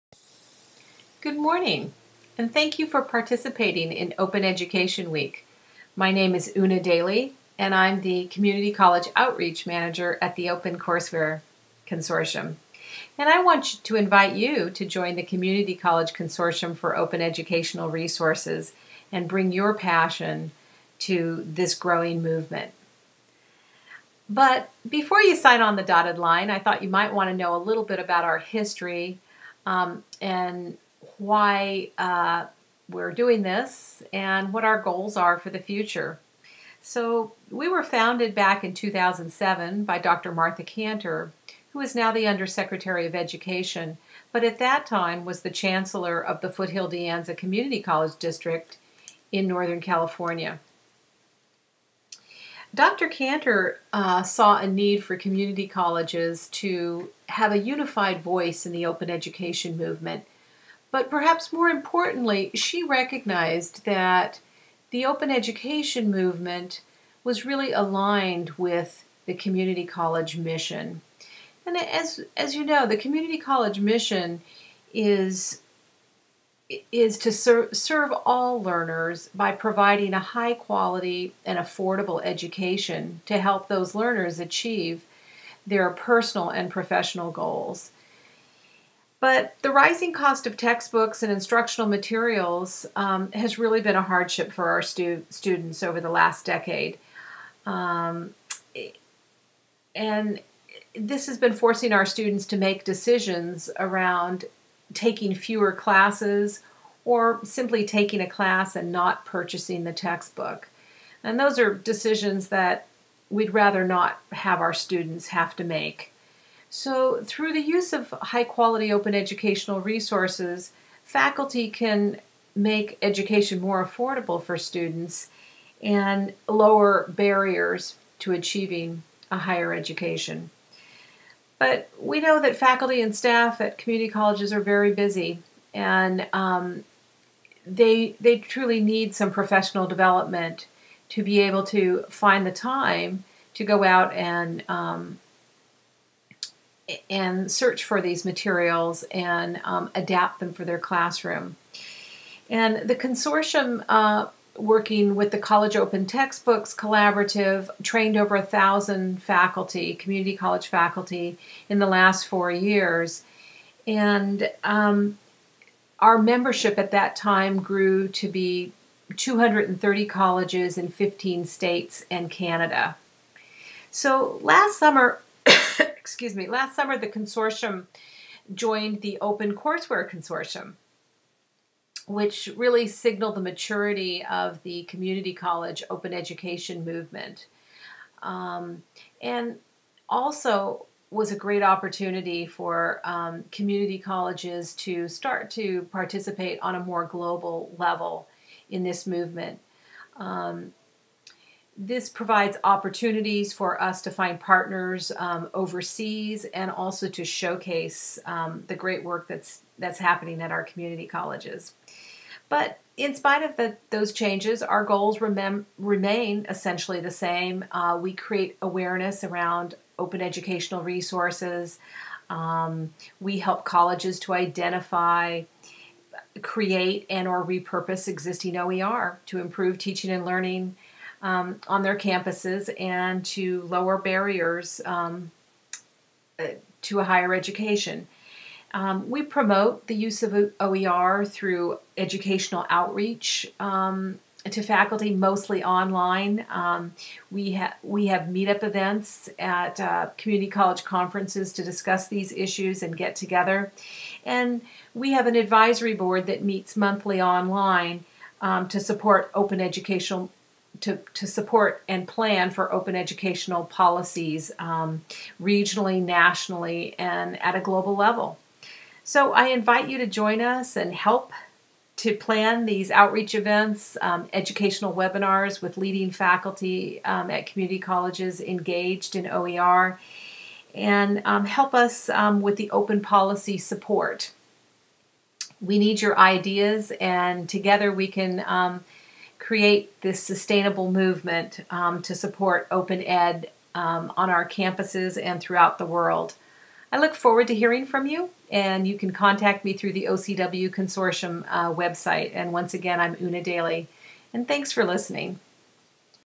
Welcome to the Community College Consortium for OER
Una Daly, Community College Outreach Manager, at the Open Courseware Consortium speaks about the history, motivation, and goals of the Community College Consortium for Open Educational Resources and invites colleges to join the consortium and build a sustainable open educational movement on our campuses and worldwide.